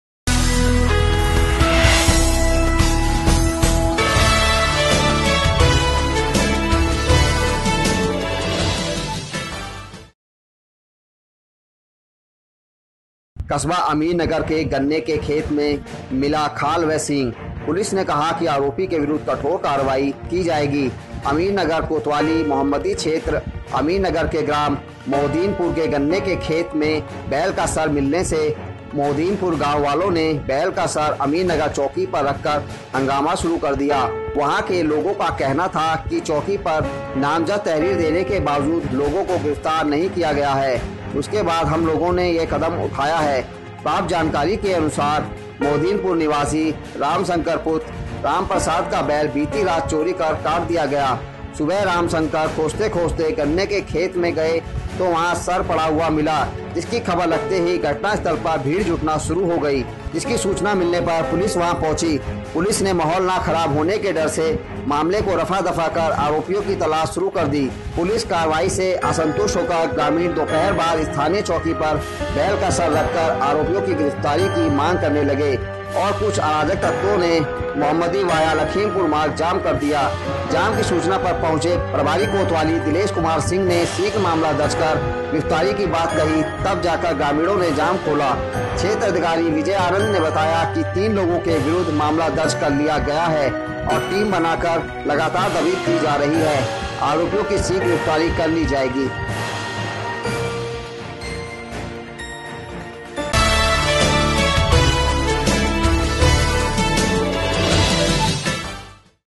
प्रतिबन्धित जानवर का शव मिलने से कस्बा मे तनाव-Today24
कस्बा अमीर नगर के गन्ने के मिली खाल व सीग|  पुलिस ने कहा कि आराेपी के विरूद्ध कठाेर कार्यवाही  अमीर नगर (खीरी) कोतवाली मोहम्मदी क्षेत्र अमीर नगर के ग्राम मोहद्दीनपुर में गन्ने के खेत में बैल का सर मिलने से मोहद्दीनपुर गांव वालों ने बैल का सर अमीर नगर चौकी पर रखकर हंगामा करना शुरू किया लोगों का कहना था कि चौकी पर नामजद तहरीर देने के बावजूद लोगों को। गिरफ्तार नहीं किया गया उसके बाद हम लोगों ने यह कदम उठाया।
प्राप्त जानकारी के अनुसार मोहद्दीनपुर निवासी राम शंकर पुत्र रामप्रसाद का बैल बीती रात चोरी कर काट दिया गया सुबह राम शंकर खोजते-खोजते गन्ने के खेत में सर ढूंढ निकाला
गांव में खबर लगते ही घटनास्थल पर भीड़ का जुटना शुरू हो गया सूचना पर पहुंची चौकी पुलिस ने माहौल ना खराब होने के डर से मामले को रफा-दफा कर आरोपियों की तलाश शुरू कर दी पुलिस कार्रवाई से असंतुष्ट होकर ग्रामीणों ने दोपहर बाद स्थानीय चौकी पर बैल का सर रखकर आरोपियों की गिरफ्तारी करने की मांग करने लगे और कुछ अराजक तत्वों ने मोहम्मदी वाया लखीमपुर मार्ग पर जाम लगा दिया जाम की सूचना पर पहुंचे प्रभारी कोतवाली दिलेश कुमार सिंह ने शीघ्र मामला दर्ज कर गिरफ्तारी की बात कही तब जाकर ग्रामीणों ने जाम खोला क्षेत्राधिकारी विजय आनंद ने बताया कि 3 लोगों के विरुद्ध मामला दर्ज कर लिया गया है टीम बना दी गई है लगातार दबिश दी जा रही है आरोपियों की शीघ्र गिरफ्तारी कर ली जाएगी।

T.V. News

Application.

views!!!

News."